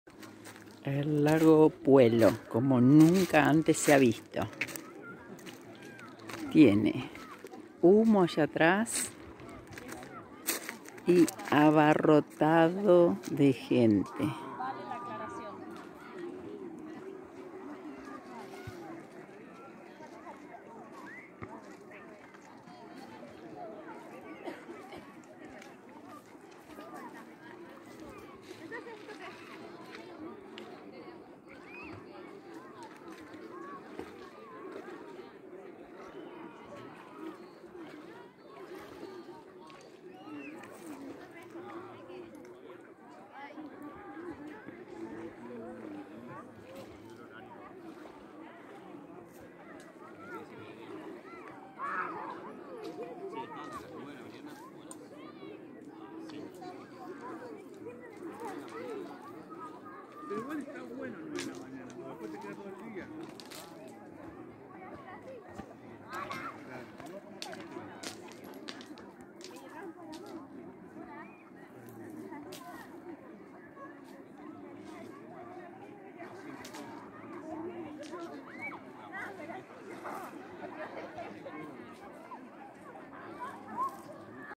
Lago Puelo
Hermoso lugar, mucho calor 4/2/21 y mucho turismo, tiene dos playas, en la que esta en la izquierda imposible ir, parecía las playas de Mar del Plata. en la otra caminas por el bosque y llegas hasta la desembocadura.